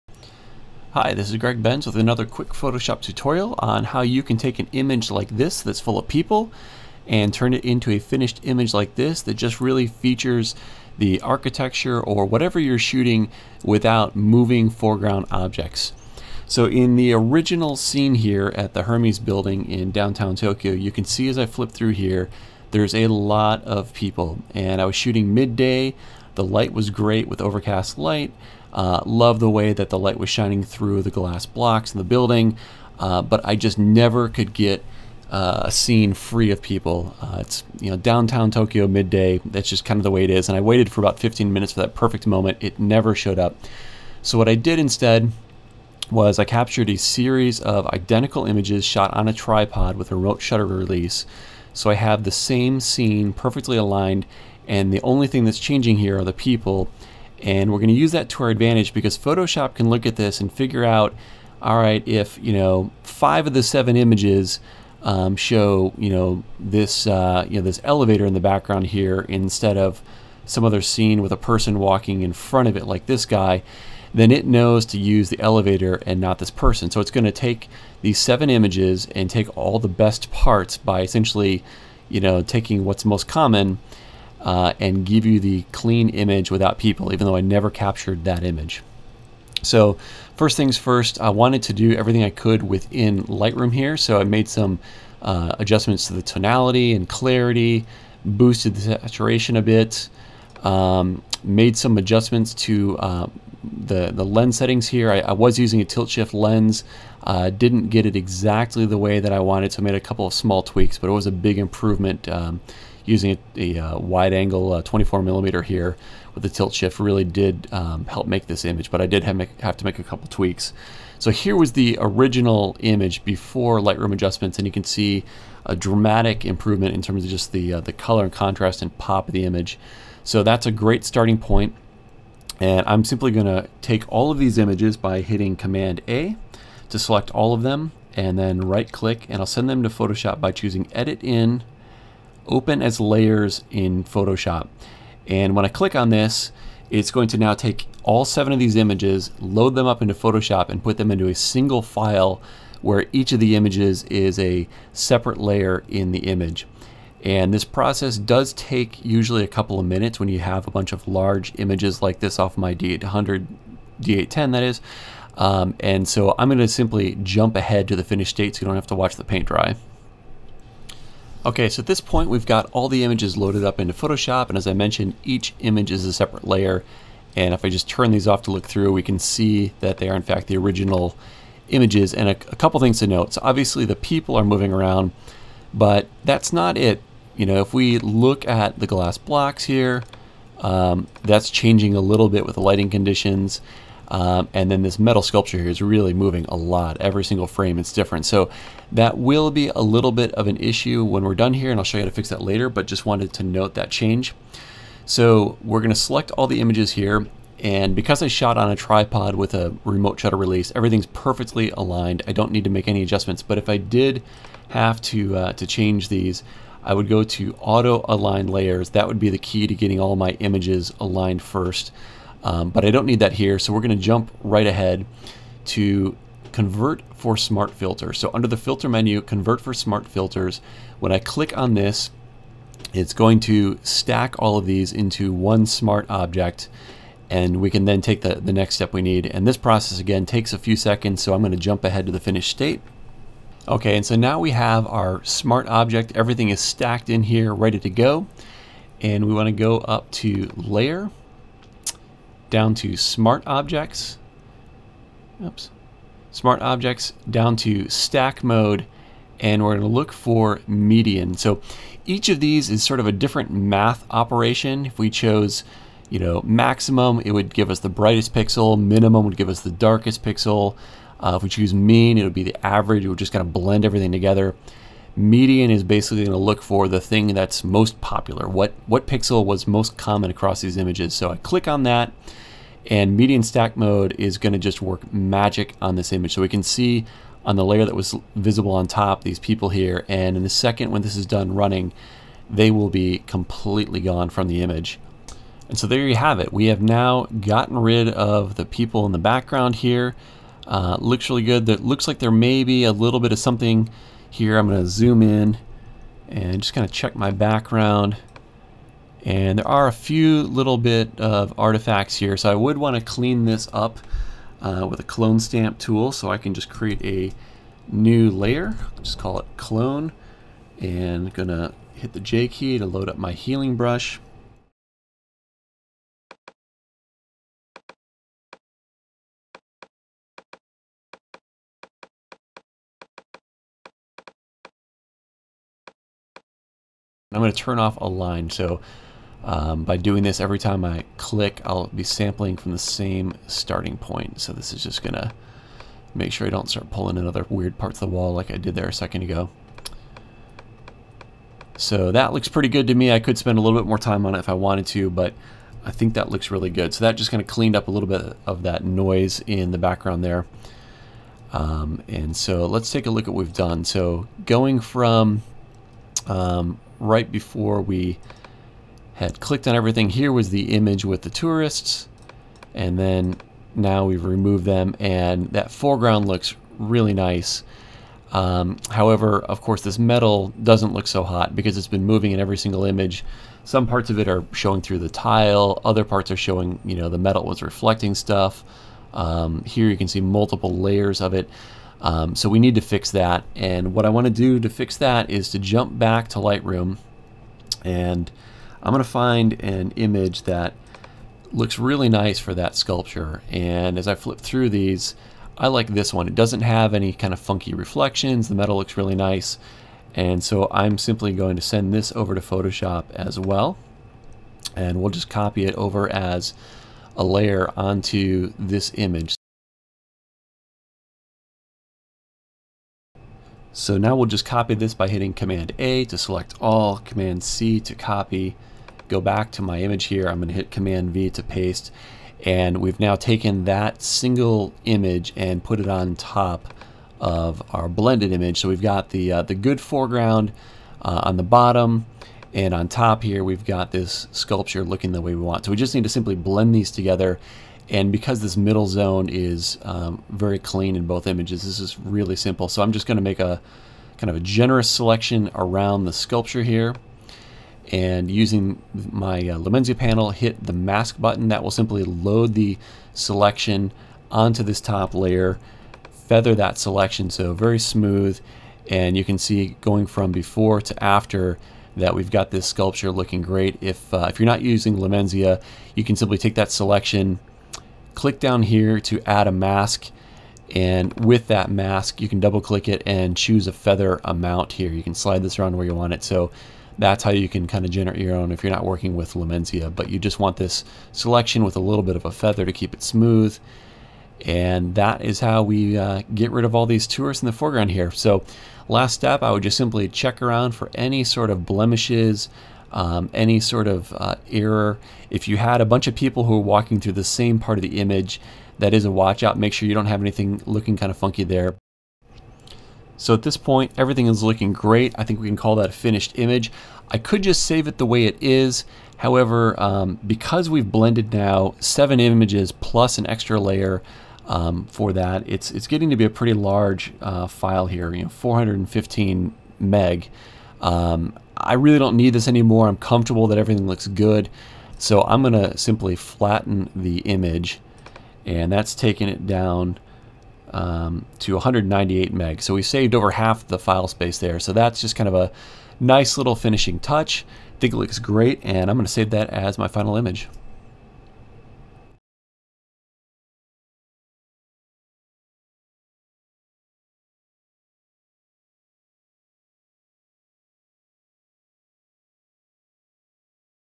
How to remove Tourists in Photoshop
Learn how to remove tourists and other moving distractions from any photo in Photoshop easily in this quick tutorial.

Key segments:
0:00 Intro & Lightroom setup
2:11 Sending the images to Photoshop as layers
3:35 Make Smart Object and set to "median" to get rid of moving objects
5:00 Clone/heal to fix artifact
6:19 Blending in original image to keep a moving object